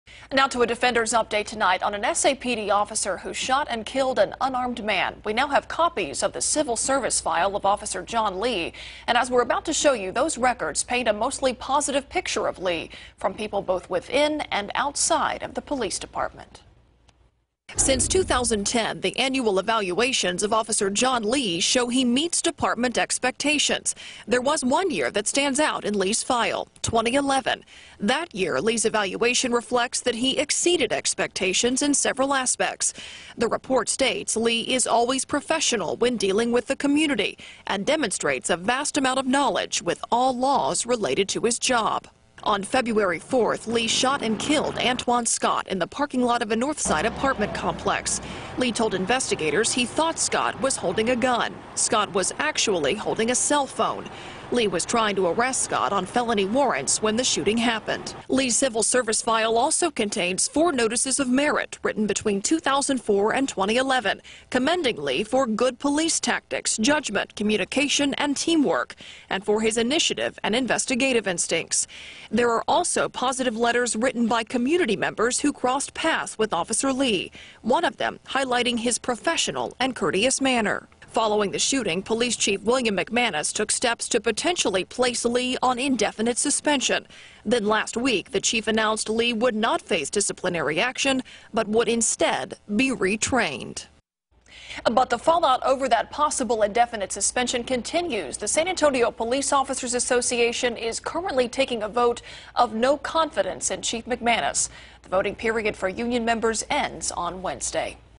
VIDEO: Officer John Lee’s civil service file paints mostly positive picture
The civil service file of San Antonio police Officer John Lee paints a mostly positive picture of the officer who shot and killed an unarmed man earlier this year.